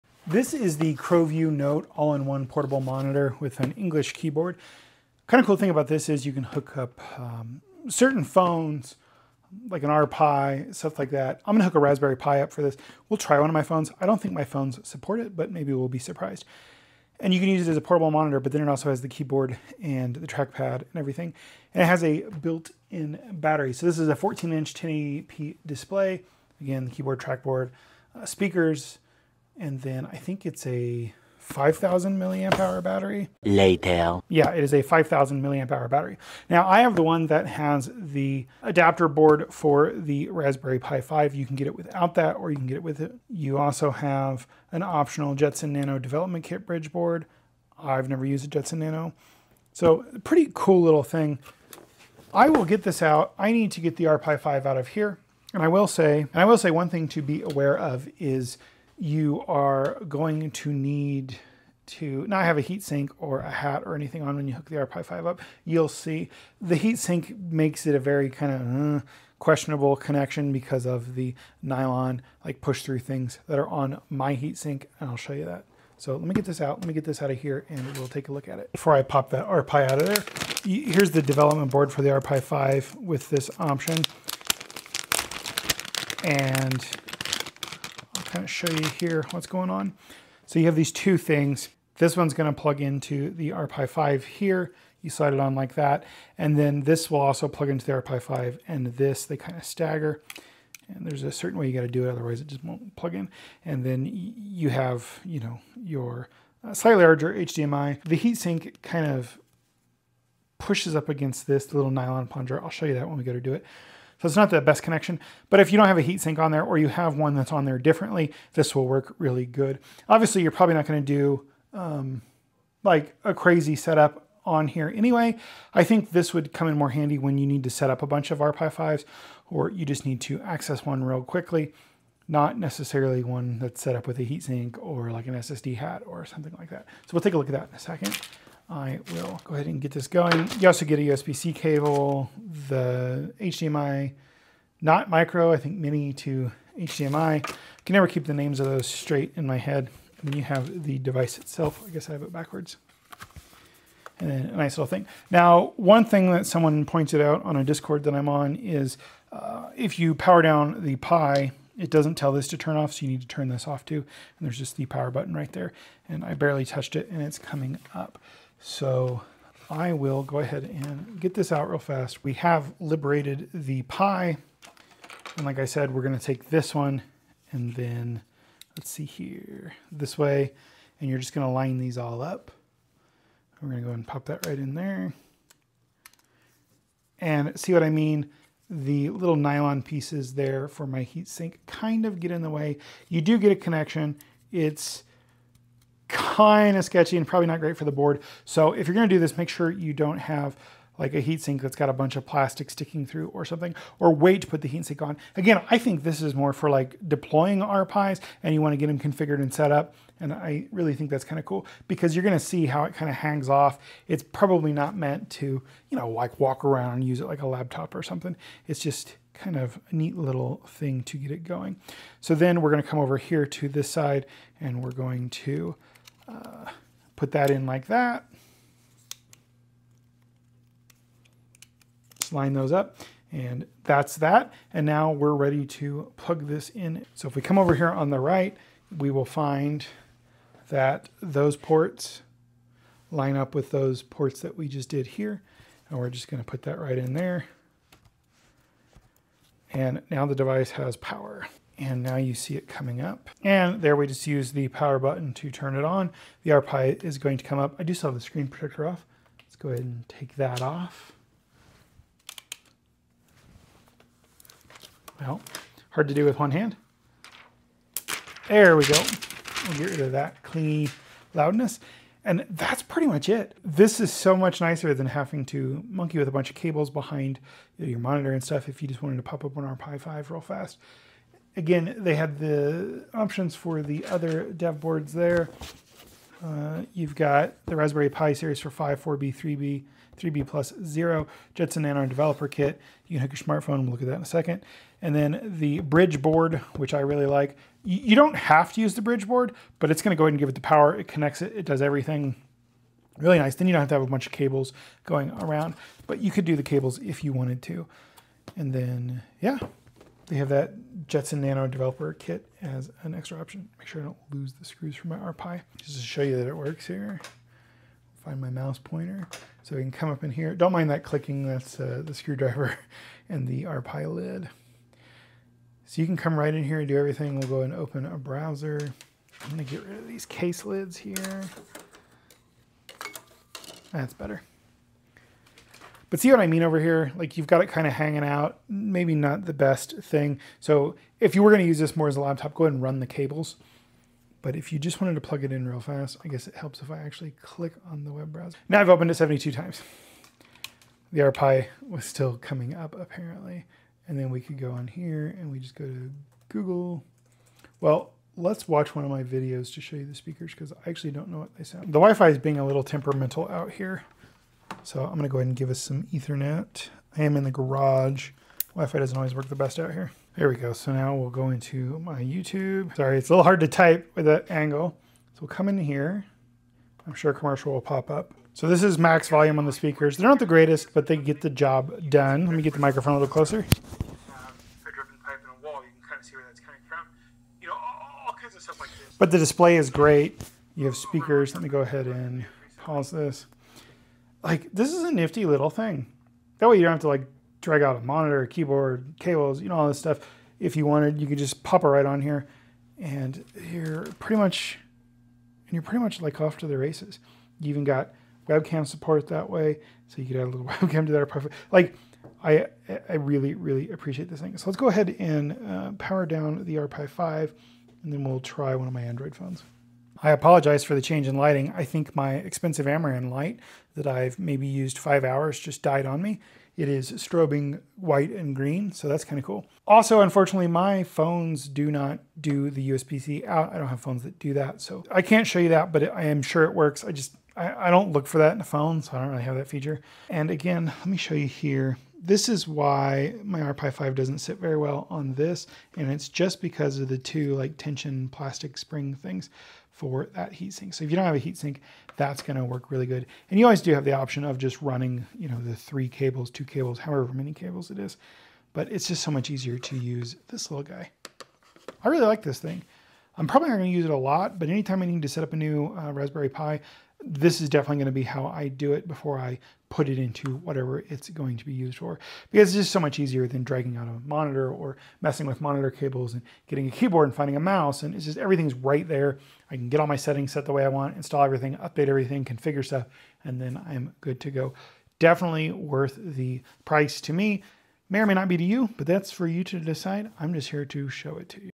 This Is How Pi 5 Goes Portable - CrowView Note Lapdock
The Lapdock
Start: 12/29/2025 12:01 AM PST
End: 01/04/2026 11:59 PM PST

The CrowView Note is a portable lapdock designed to turn devices like the Raspberry Pi 5 into a usable, laptop-style setup. Instead of being a full computer, it provides the screen, keyboard, trackpad, battery, and ports, letting your existing hardware do the computing.

With the Raspberry Pi 5 adapter, this lapdock becomes a compact and practical way to work with Linux, development projects, networking tools, and portable SBC setups without carrying a full monitor and keyboard. It’s built for flexibility, portability, and clean cable management, making it especially appealing to Raspberry Pi users and tinkerers.

If you’re looking for a simple way to make your Pi more usable on the go, this lapdock offers a straightforward and purpose-built solution.